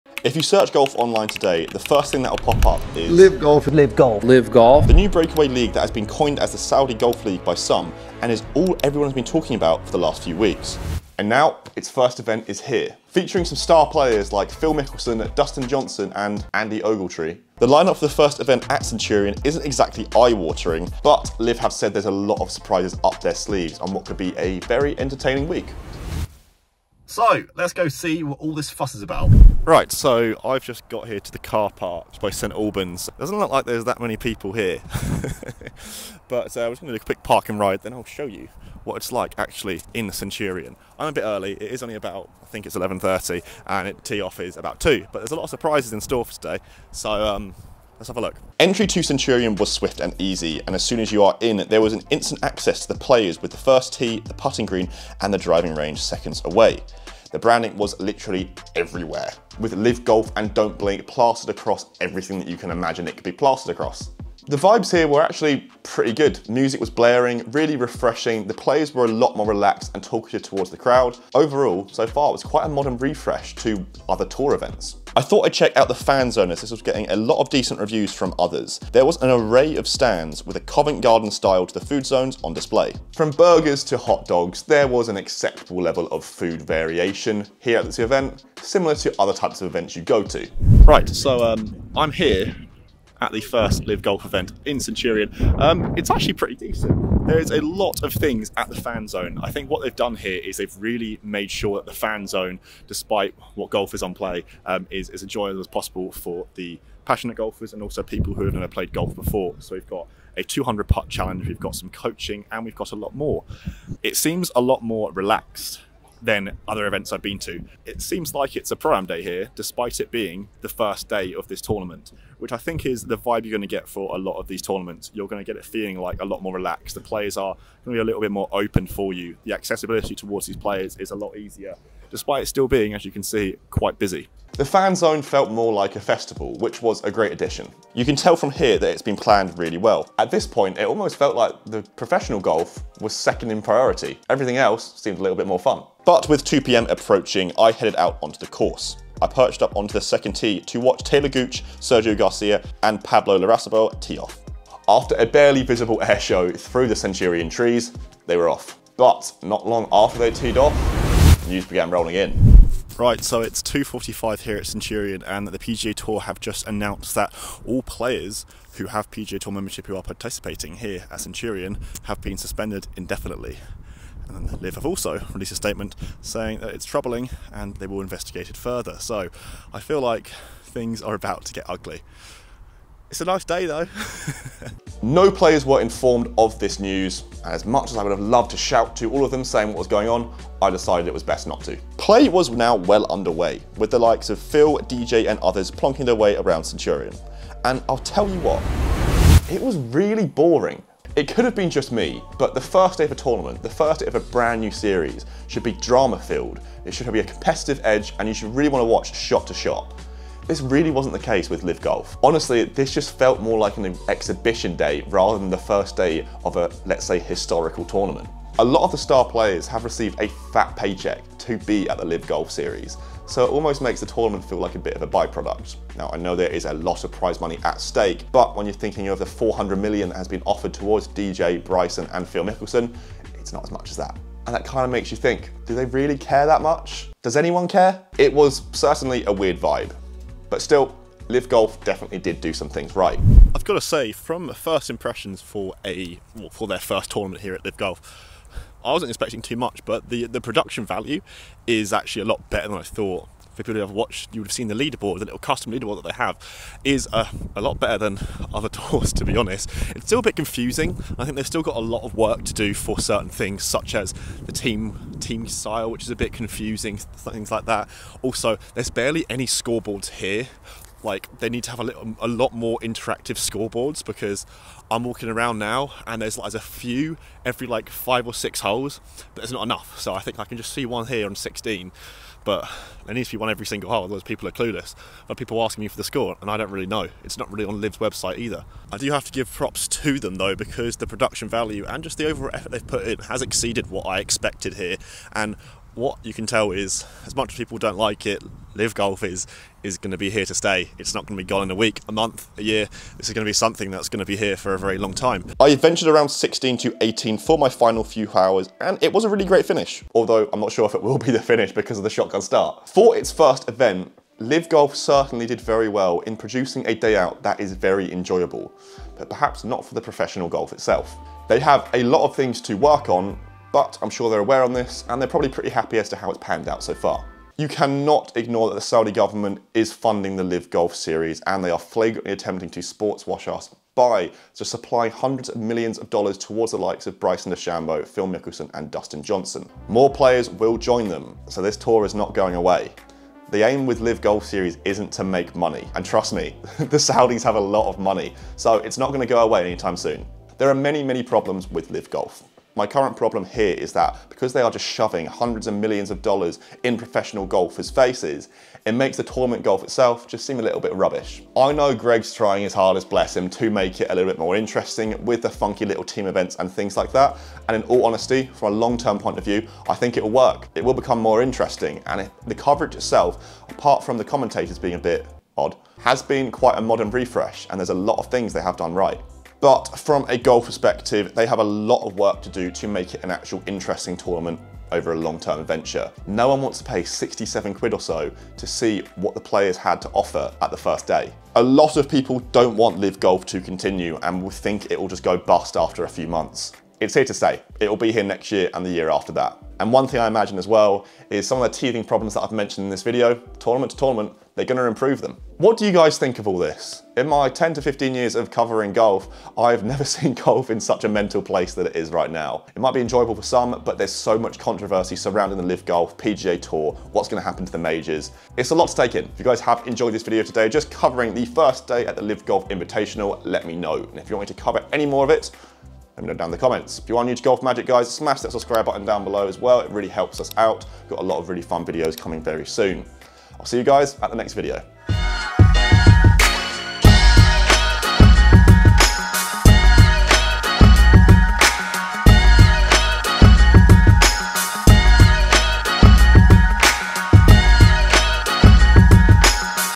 I Went To The First Day Of The LIV Golf Series at Centurion Club
LIV Golf is all the rage in the game right now, so we headed over to the first LIV Golf tournament at Centurion Club in Hertfordshire to see what all the fuss was about!

Some bits were great, but the rest of it... well, not so much.

How does LIV Golf compare to PGA Tour and DP World Tour events?

Watch our LIV Golf Vlog to see what GolfMagic's Alex Lodge makes of it.

GolfMagic.com is the #1 online golf website for news, reviews, tips and instruction with more than 3 million monthly users and growing.